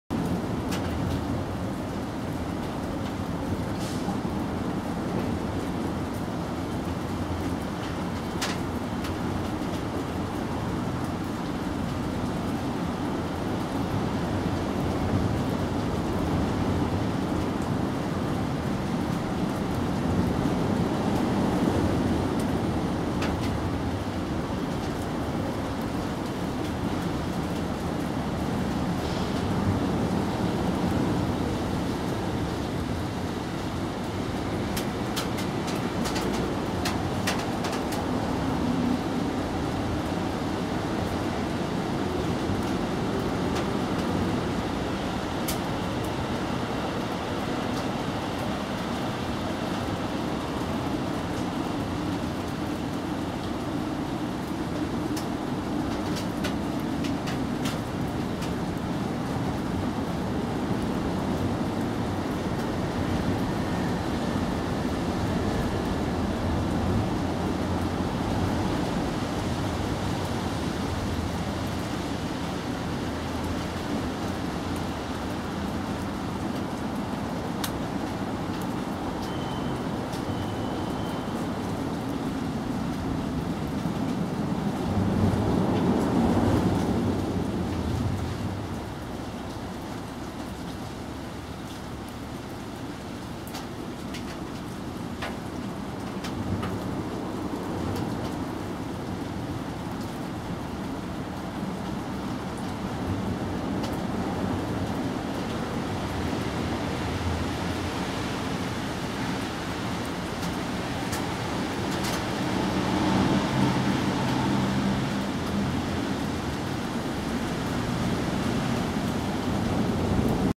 ねむしま子猫、母猫の足元に kitten and mother
ねむしま子猫、お乳を飲み終え、母猫の足元にピッタリくっついています。